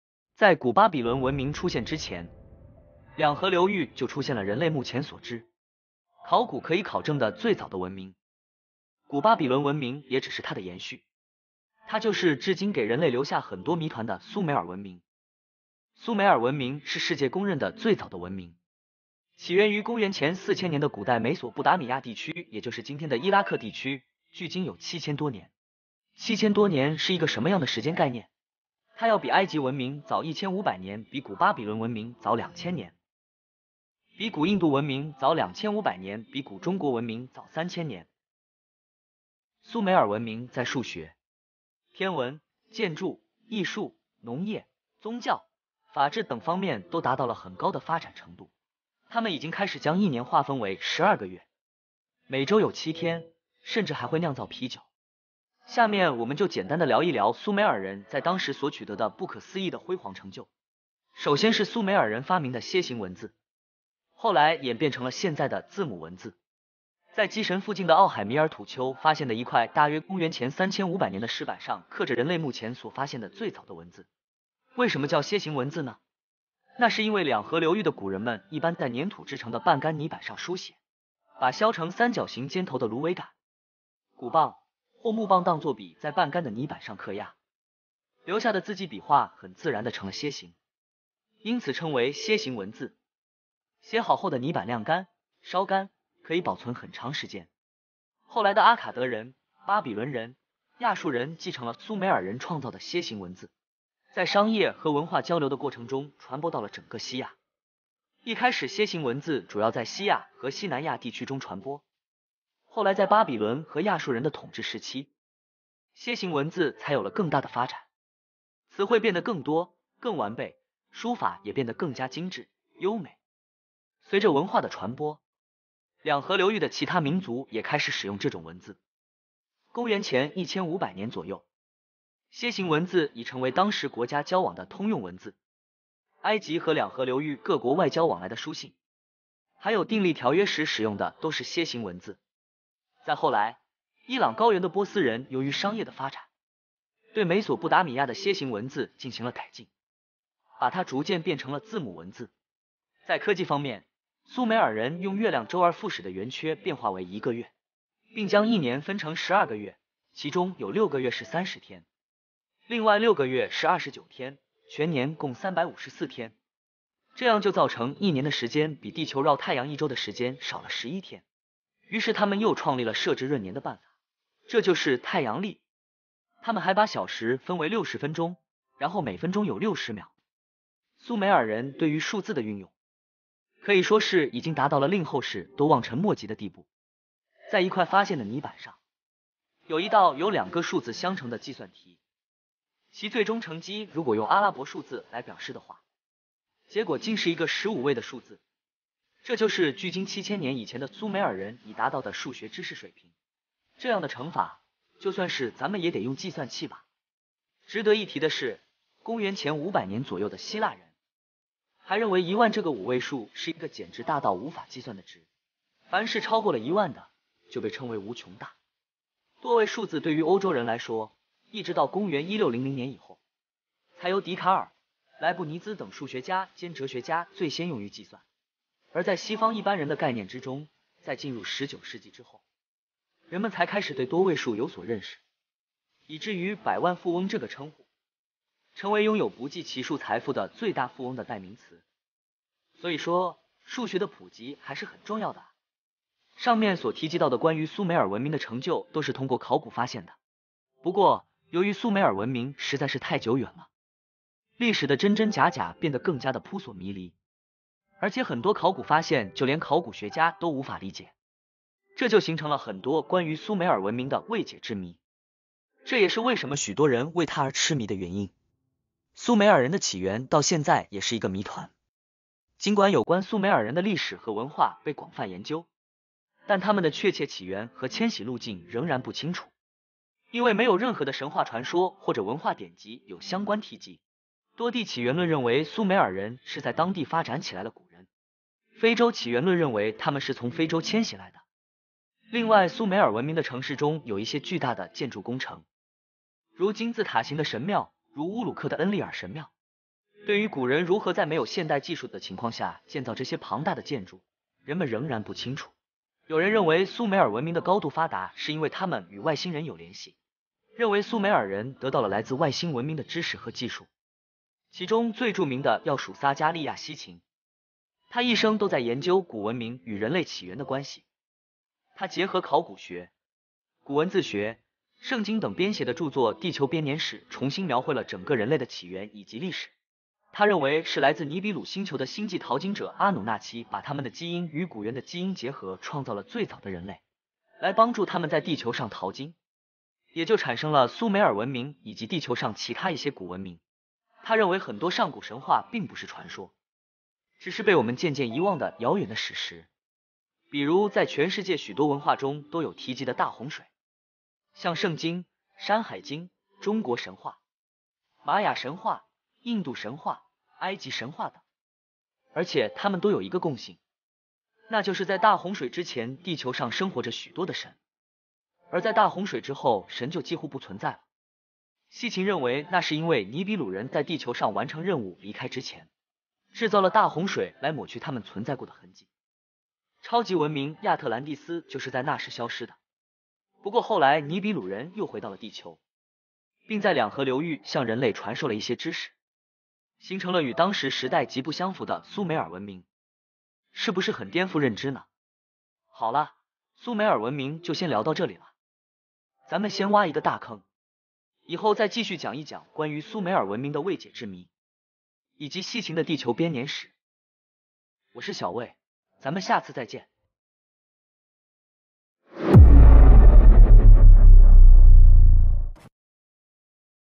蘇美爾文明，神/外星人存在的證據?
蘇美爾，一個與時代極不相符的高度發達的人類文明，雖然有很多的考古證據證明蘇美爾人曾經真實的存在，但是仍舊有太多考古學家都無法理解的未解之謎。它的存在是一場歷史的意外，還是在向我們揭示另一個可能的人類歷史？